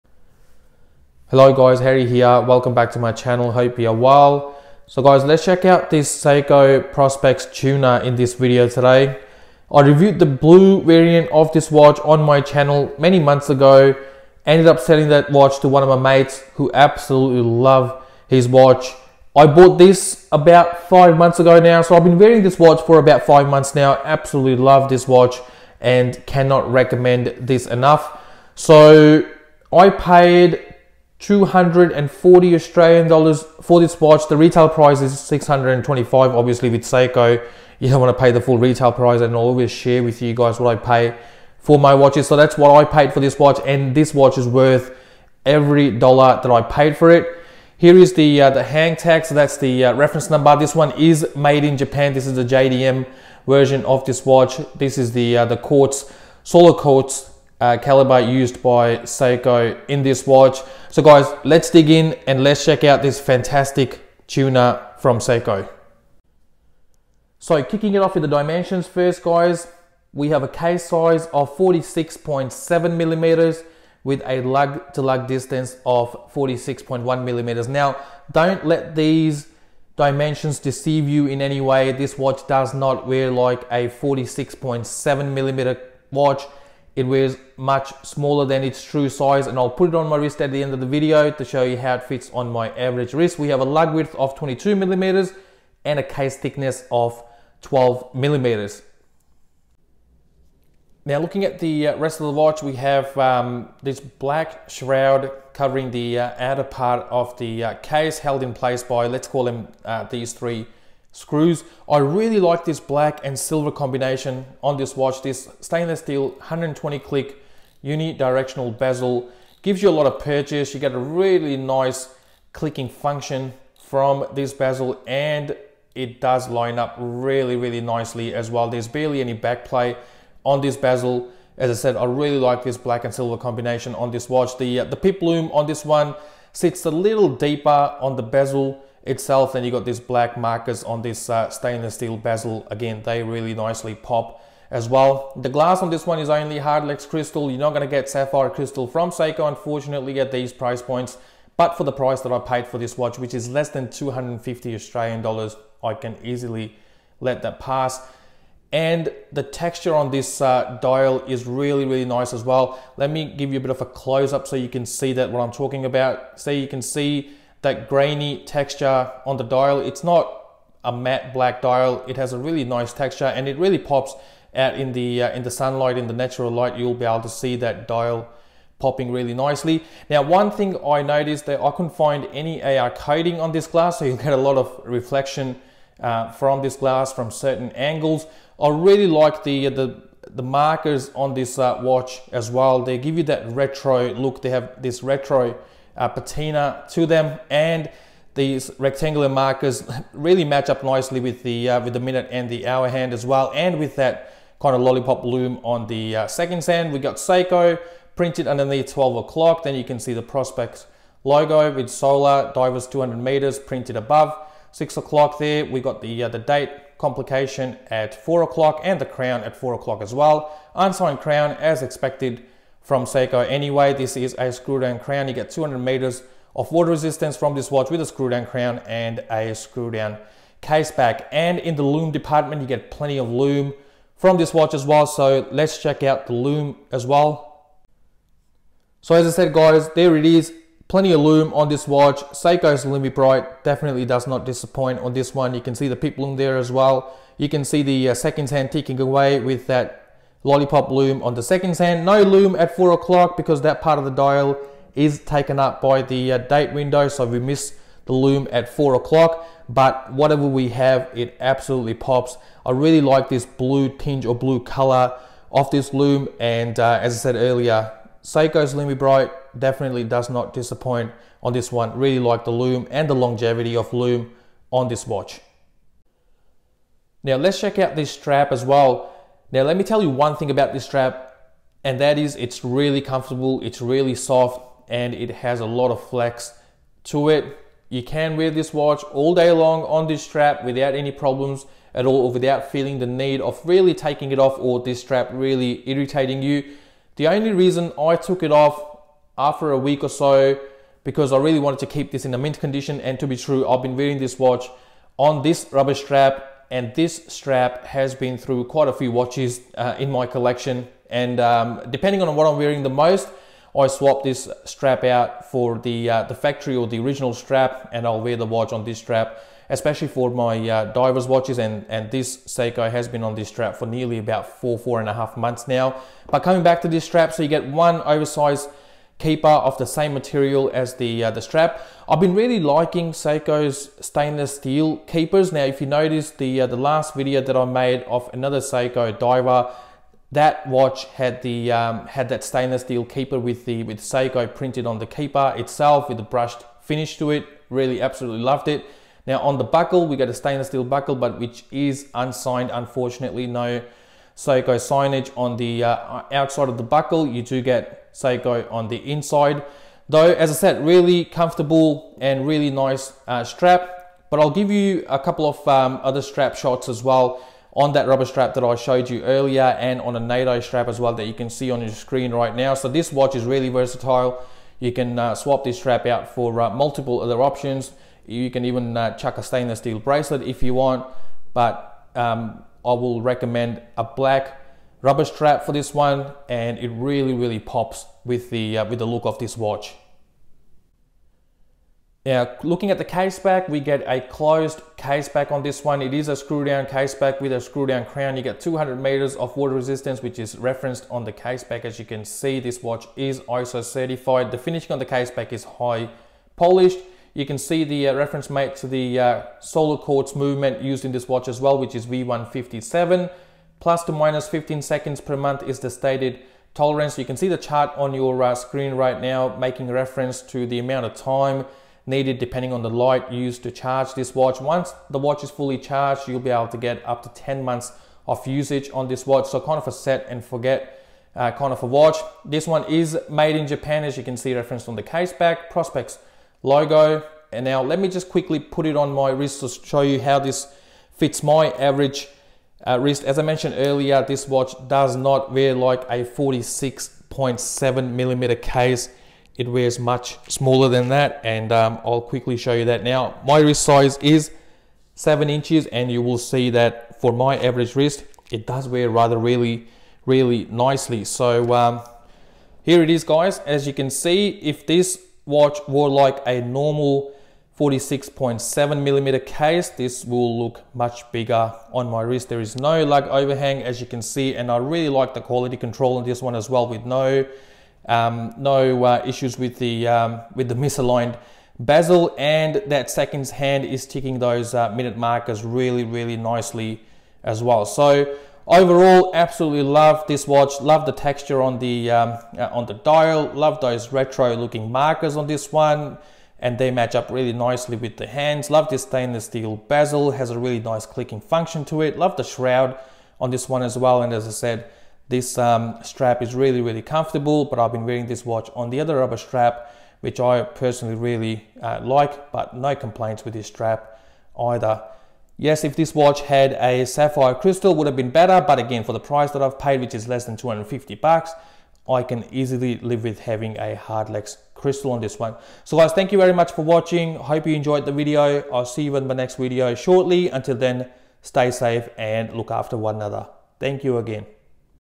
Brilliant Seiko Prospex Tuna JDM SNE541J Solar Diver
Hey guys, in this video, let's check out this fantastic Baby Tuna Diver from Seiko which is not all about the looks, but is purpose built for diving.

Let's Talk Watches.